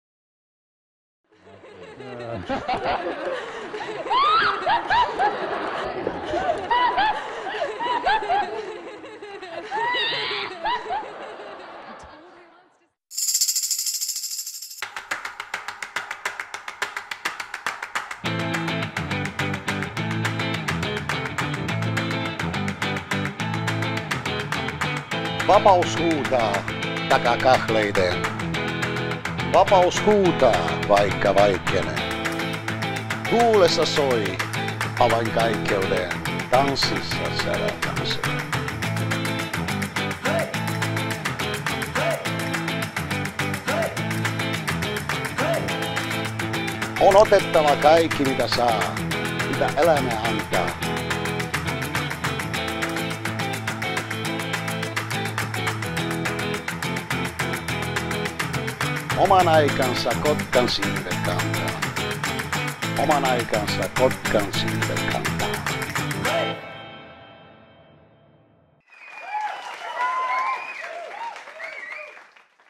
Flamenco
vapaus huutaa - minä vaikenen.
Krasie Naydenov lausuu Jalosen tekstin,
jonka sävelistä ja soitannasta vastaa Jarno Huhtanen.